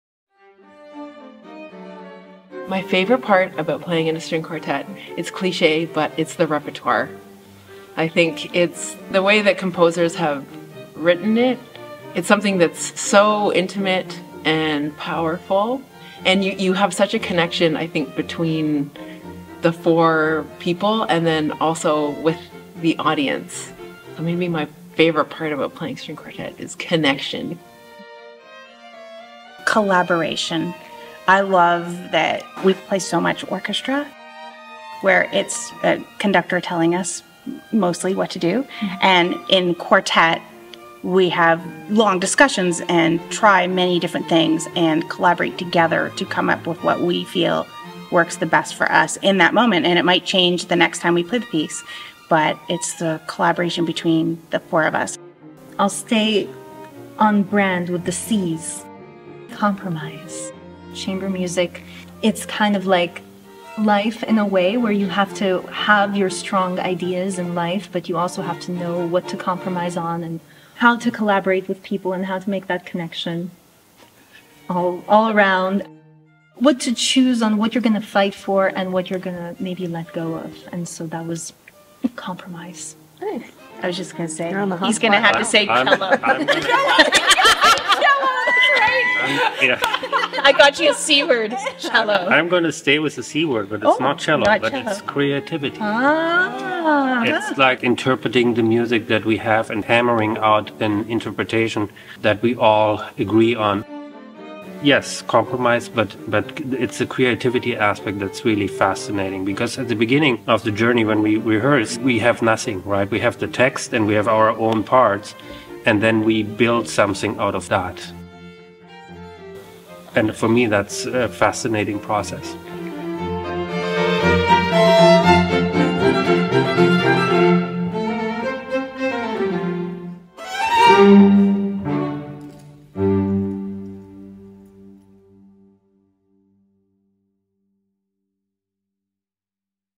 The Isabel Quartet: The C-Words
Meet the Isabel Quartet as they reflect on the importance of "C-Words" when making music together as a group.

Katya Poplyanksky - First Violin
Julia McFarlane - Second Violin
Caitlin Boyle - Viola
Wolf Tormann - Cello